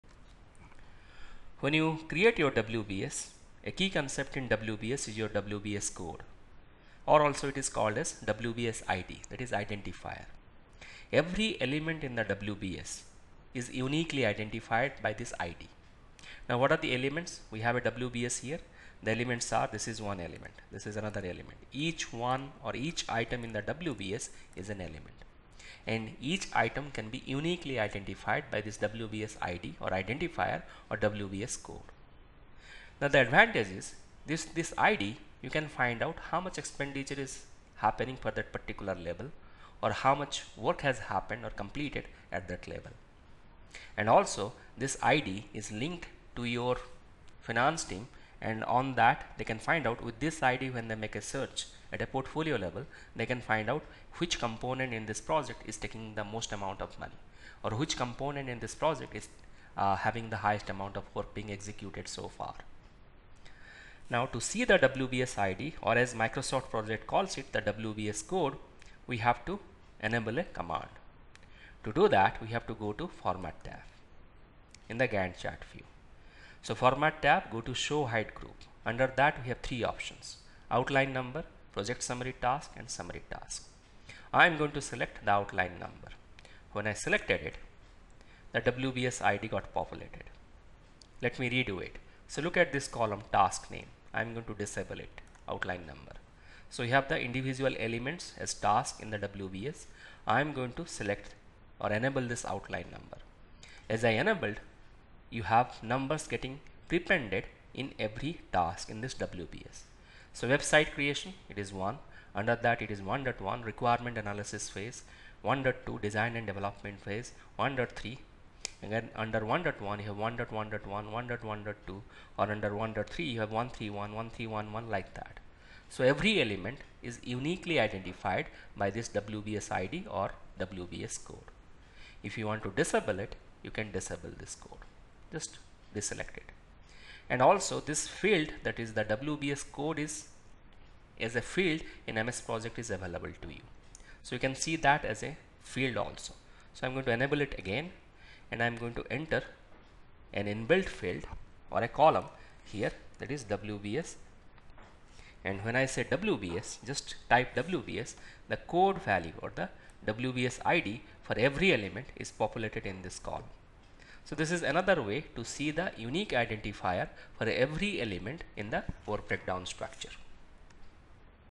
MANAGEMENT YOGI: MSP LIVE LESSONS - Guaranteed Learning: Video 3.17 Default WBS Codes (Tasks)
MANAGEMENT YOGI: MS PROJECT 2016 LIVE LESSONS - Guaranteed Learning: Video 3.17 Default WBS Codes (Tasks)
This belongs to Lesson - 3: Tasks